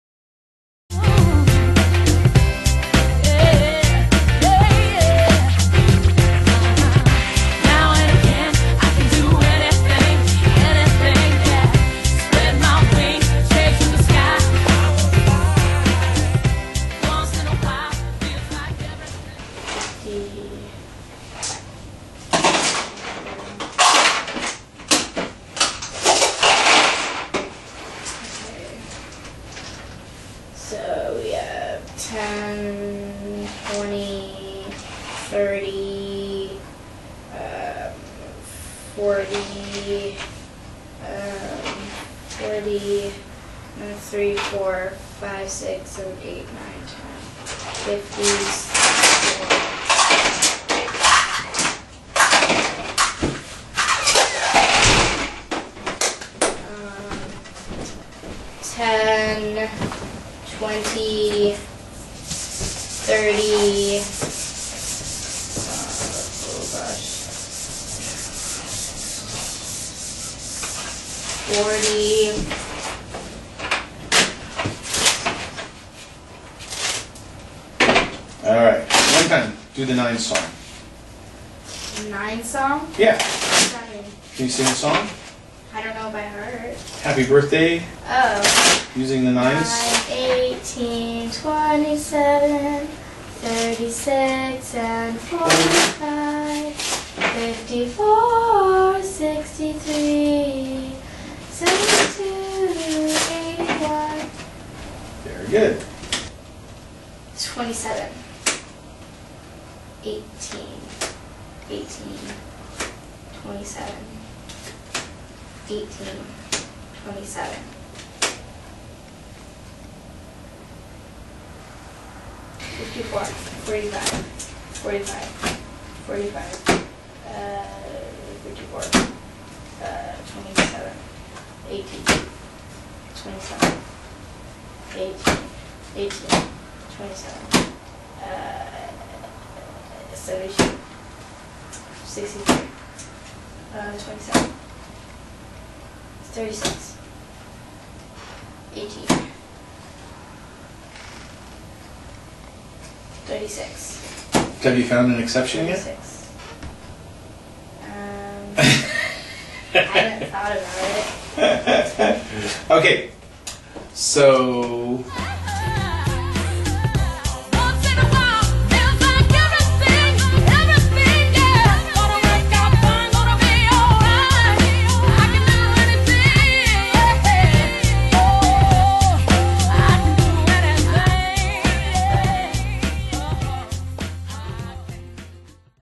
Math Games
Math Games is a short excerpt from our Math Made Easy Workshop. Includes a examples of easy and fun ways to improve math skills.